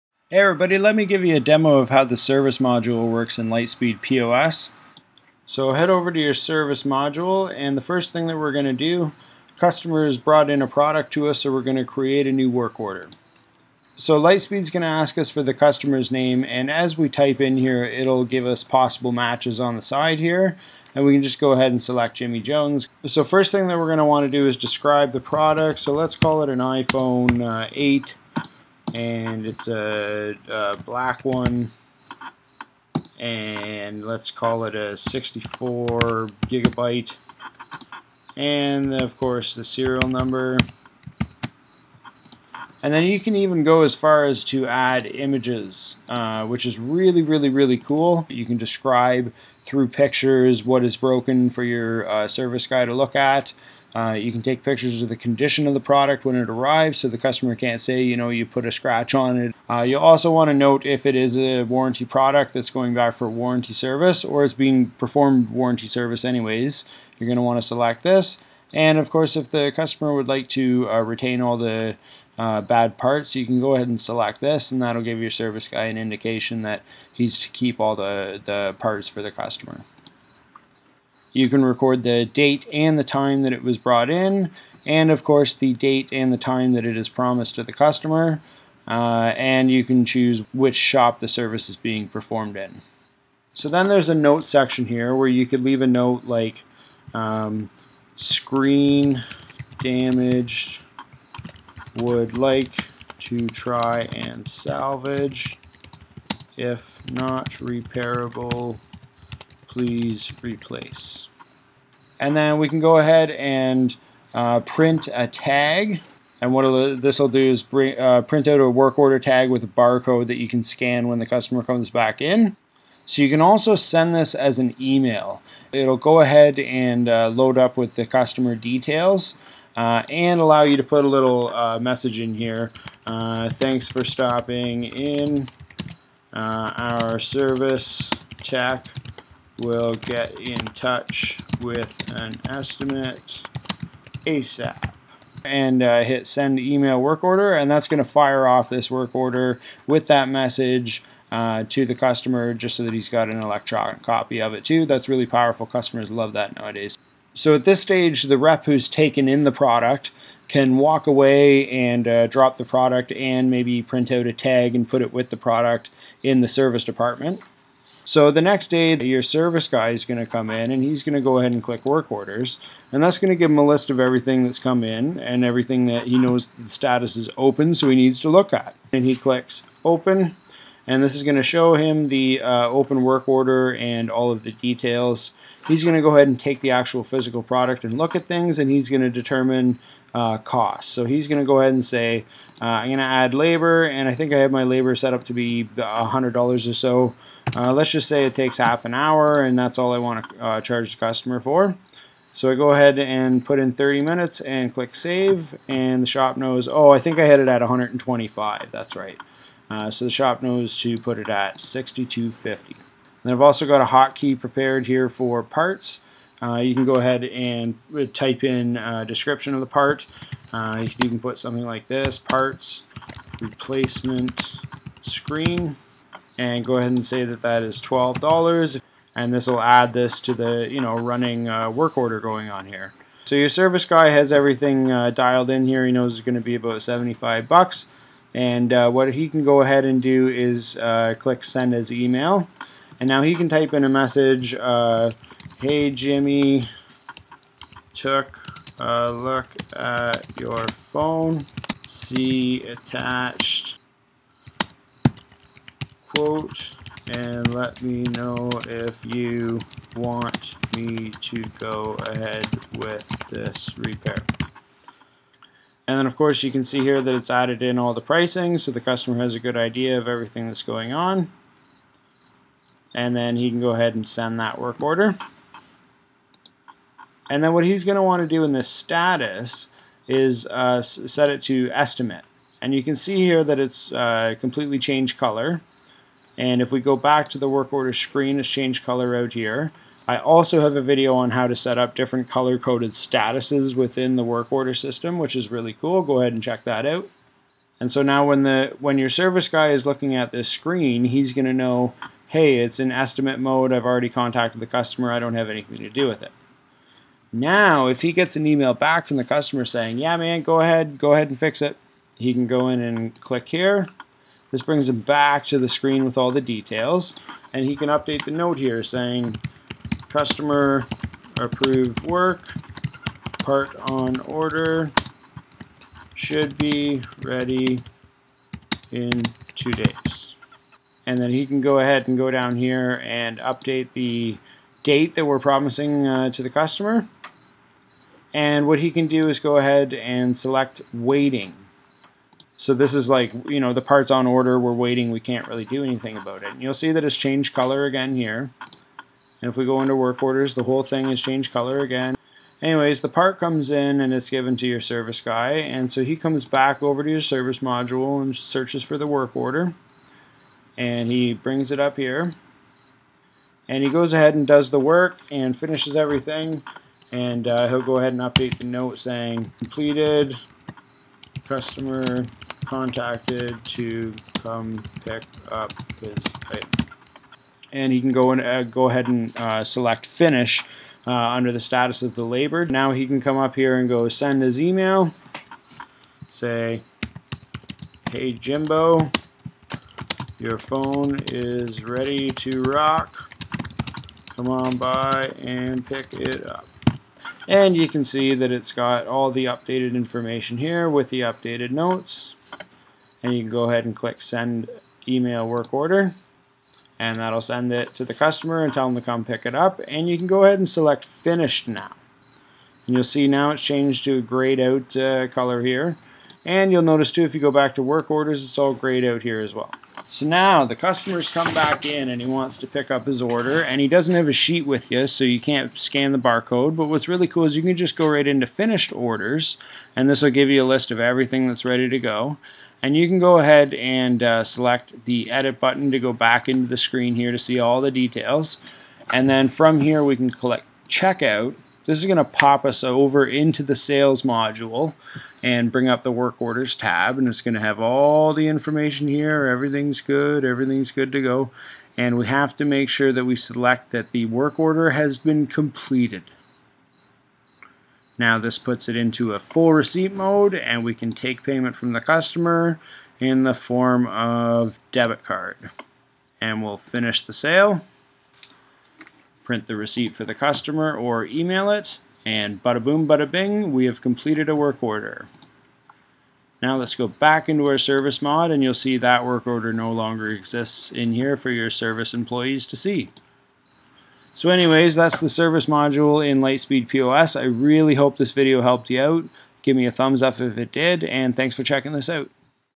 Work Orders and Service Module in Lightspeed POS - A Full Demo
In this video I'll take you through a full example of taking in a broken product for service or repair using Lightspeed POS' Service Module.

I'll give you a step-by-step demo from the point of taking in the repair item, to estimating the service, to ordering the parts and completing the repair.

We'll look at completing the sale using checkout once the repair is complete.

I also explain the benefits of the color-code "status" system within the Service Module.

If there is ANYTHING that you are confused by or that you are having trouble with, don't hesitate to get in touch! I'm always here to help.